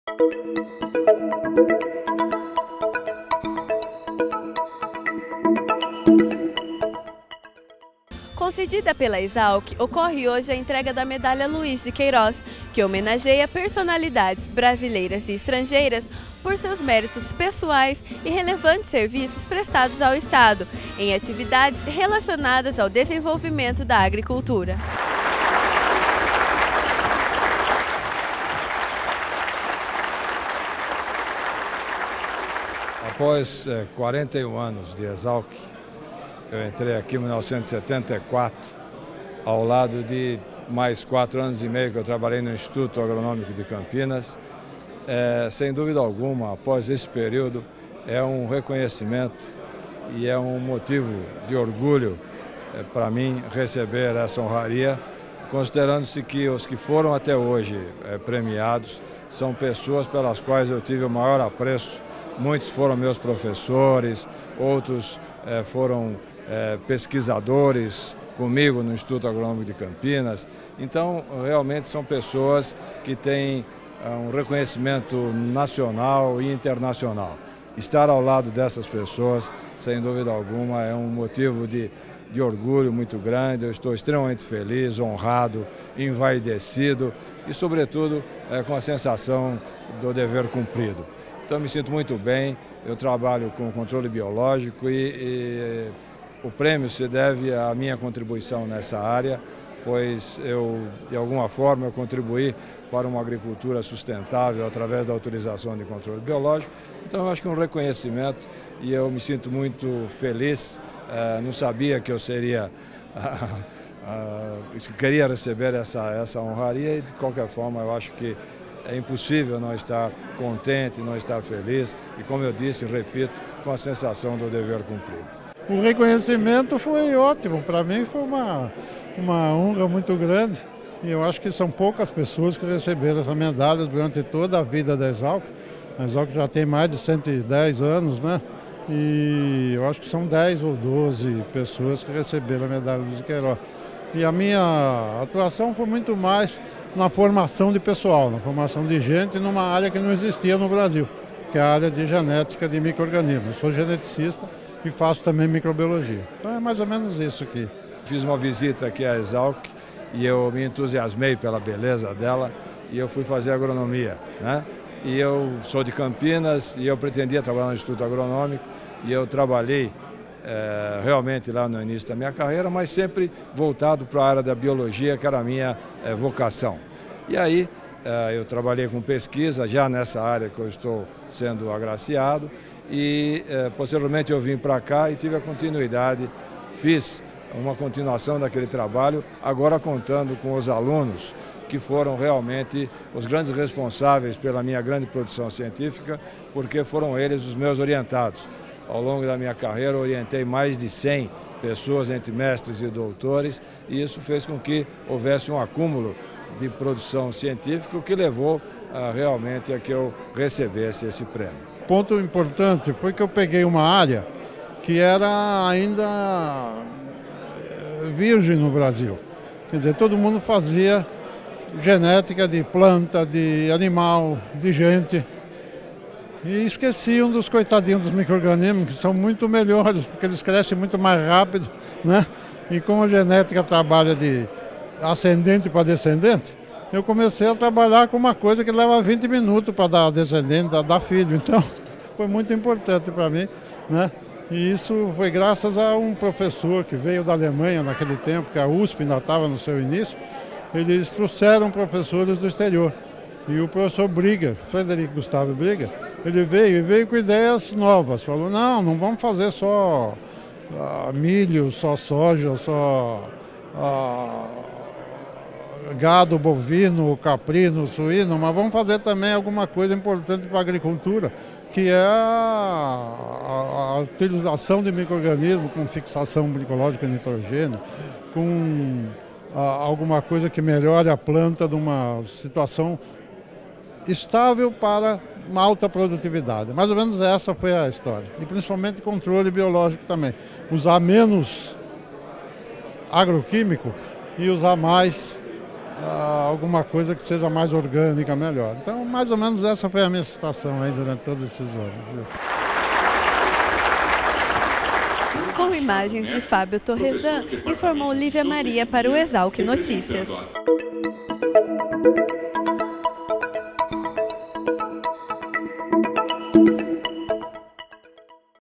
ESALQ Notícias 26/2015 - Outorga da "Medalha Luiz de Queiroz"
A Escola Superior de Agricultura Luiz de Queiroz, concedeu em 15 de janeiro, a "Medalha Luiz de Queiroz", que contempla personalidades por seus méritos pessoais e serviços prestados ao Estado, no desenvolvimento da agricultura.